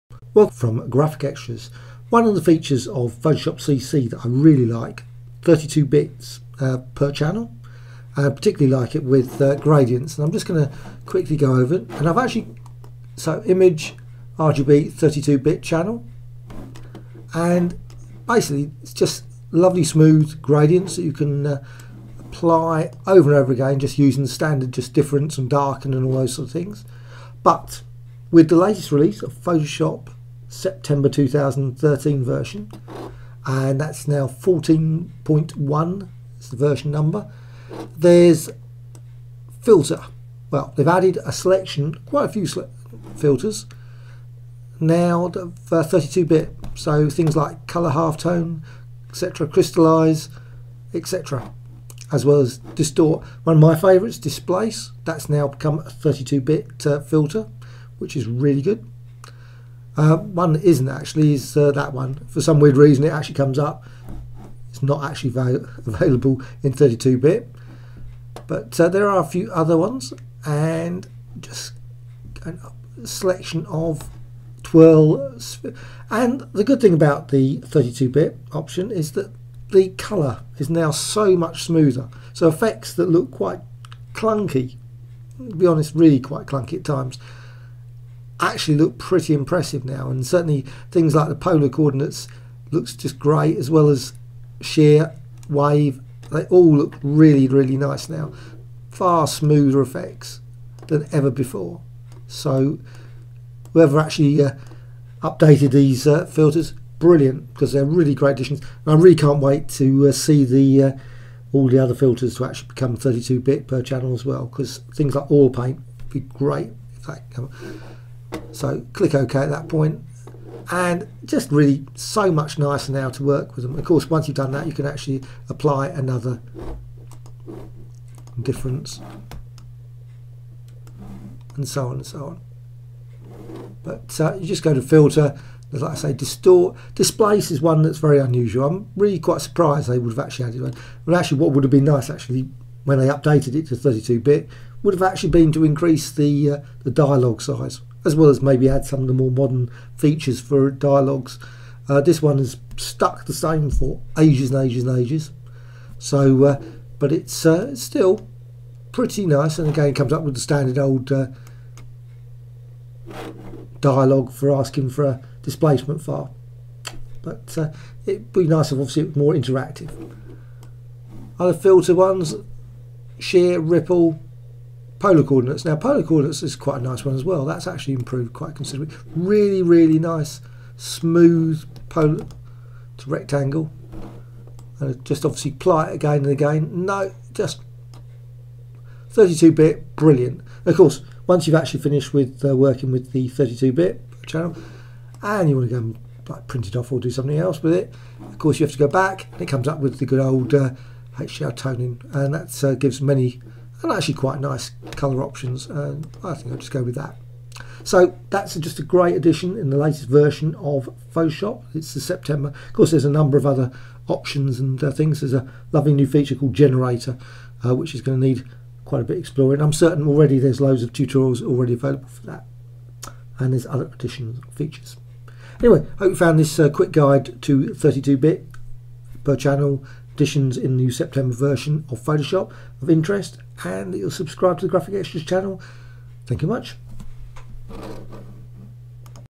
Photoshop CC Update September 2013 - 32 bits per channel and new filters
A quick guide to the new update for September 2013 of Photoshop CC and the addition of a number (20 or so) new filters. Smoother color and wonderful effects combined in particular with gradients in 32bits per channel